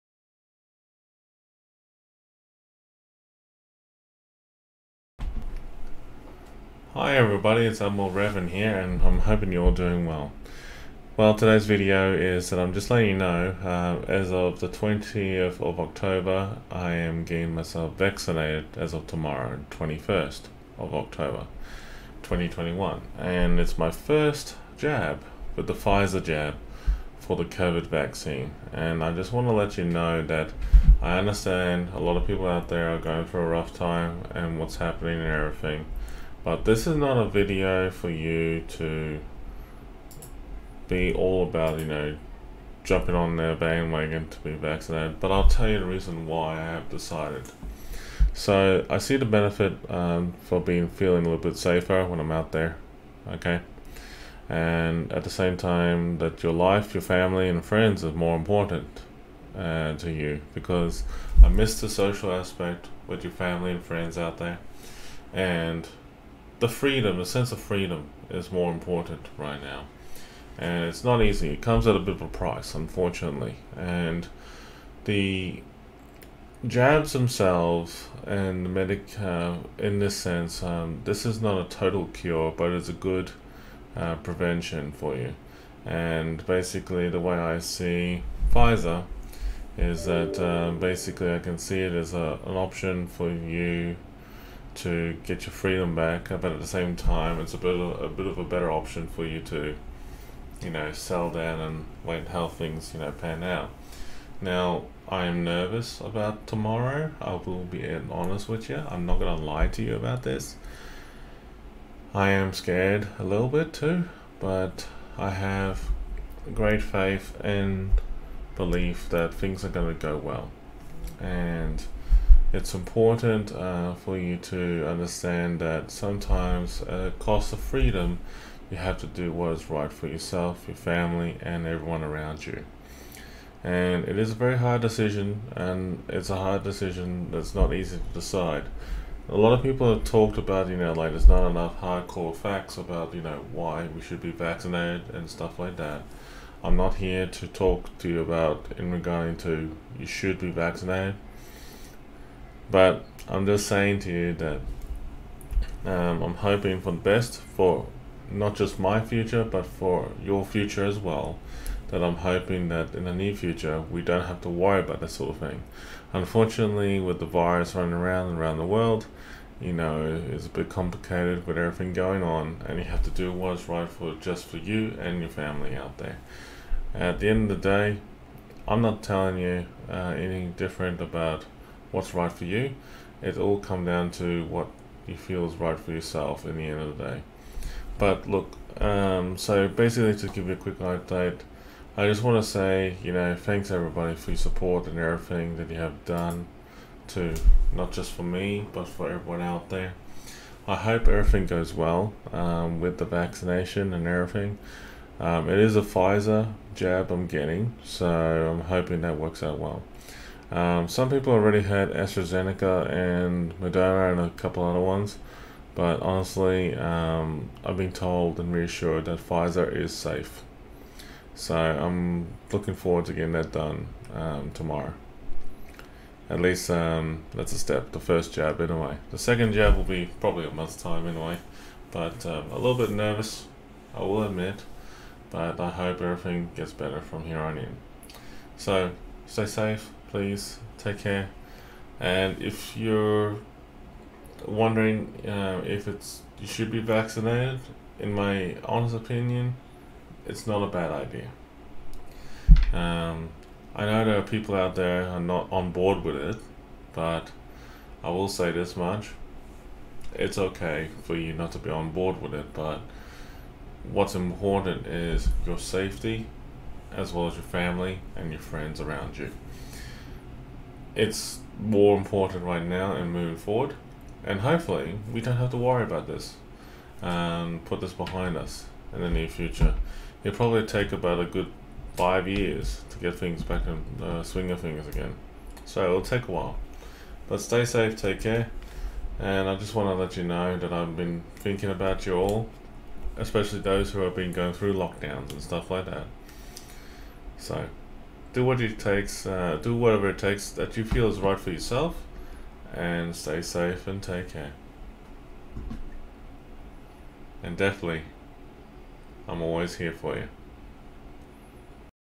Admiral Revan quick update - we are here for you
Getting vaccinated tomorrow for the first time, hope it goes well. First step into freedom